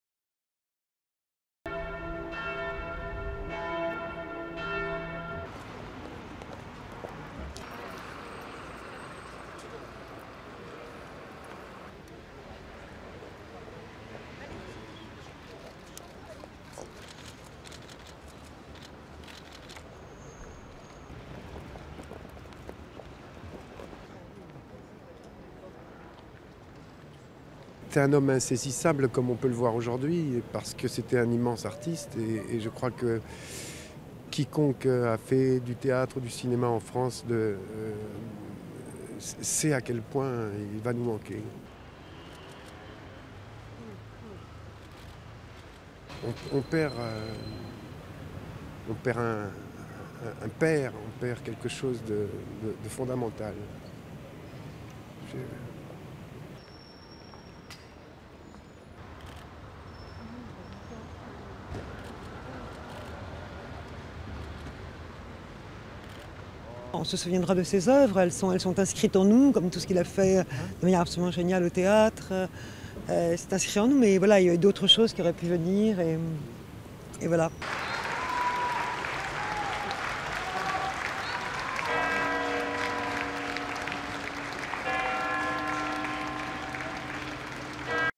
Le monde de la culture aux obsèques de Patrice Chéreau
Le monde de la culture et de la politique est venu en nombre mercredi 16 octobre pour rendre hommage à Patrice Chéreau, lors des obsèques célébrées à l'Eglise Saint-Sulpice à Paris du géant de la scène européenne décédé le 7 octobre à 68 ans. Durée: 01:28